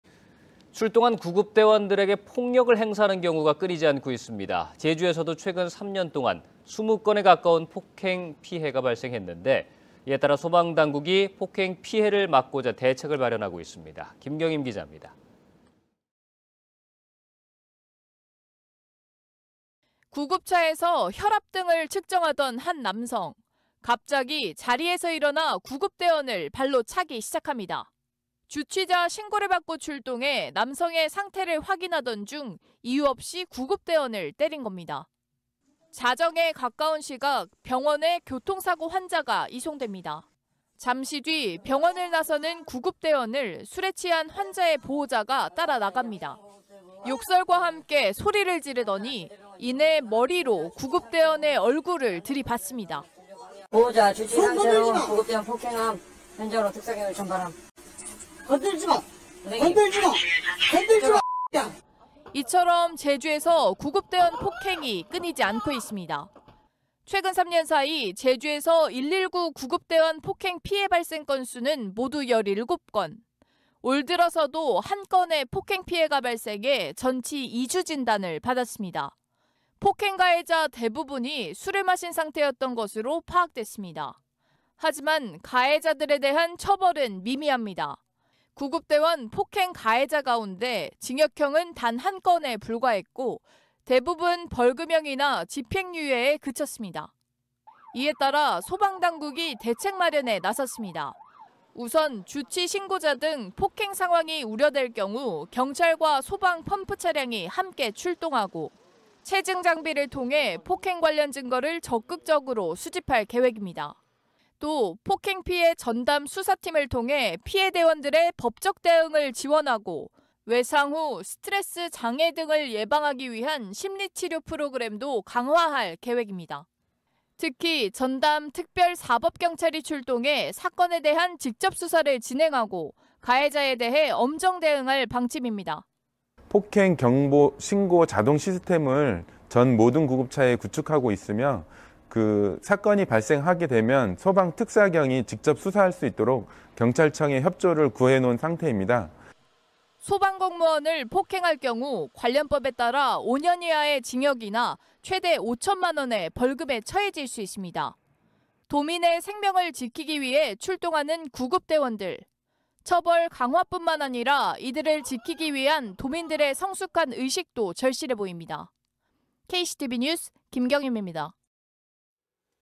119 구급대원 폭행은 '중대범죄'
출동한 구급대원들에게 폭력을 행사하는 경우가 끊이지 않고 있습니다.

제주에서도 최근 3년 사이 20건에 가까운 폭행 피해가 발생했는데요.

이에 따라 소방당국이 폭행 피해를 막기 위해 대책 마련에 나섰습니다.